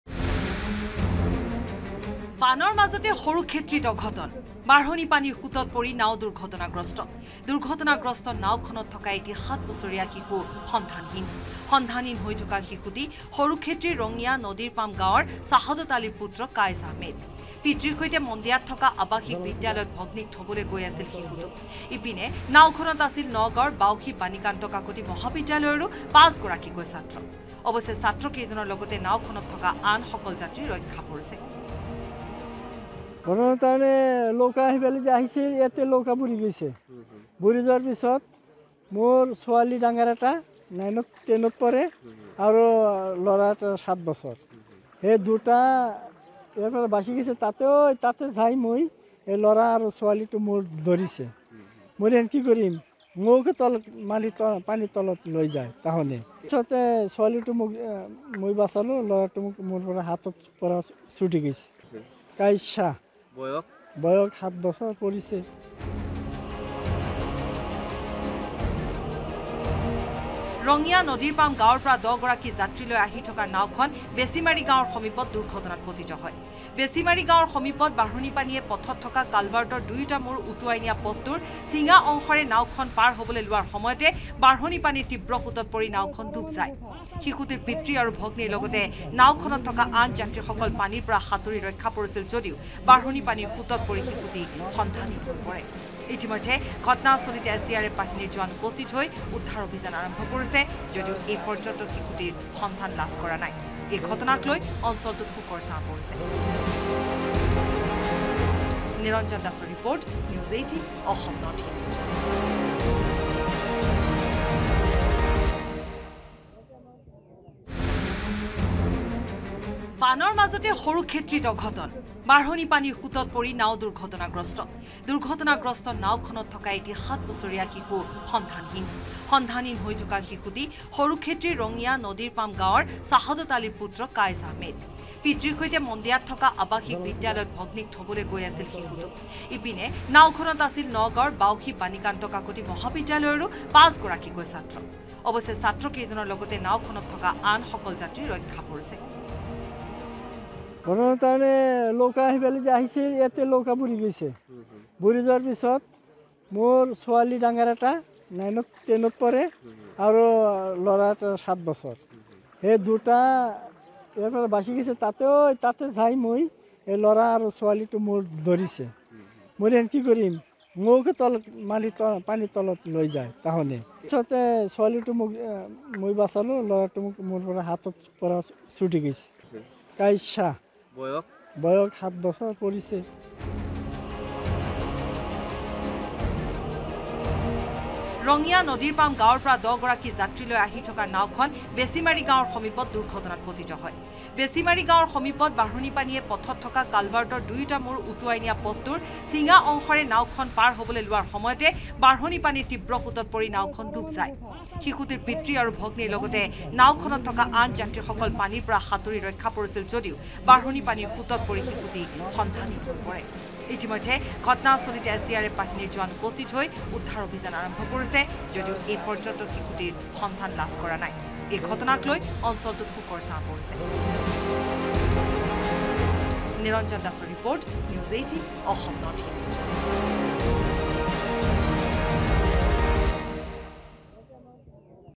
Barpeta News | বানৰ মাজতে সৰুক্ষেত্ৰীত নাও দুৰ্ঘটনা, বাঢ়নী পানীৰ সোঁতত পৰি নাও দুৰ্ঘটনাগ্ৰস্ত N18V
লগতে চাওঁক দিনটোৰ বহু গুৰুত্বপূৰ্ণ খবৰ |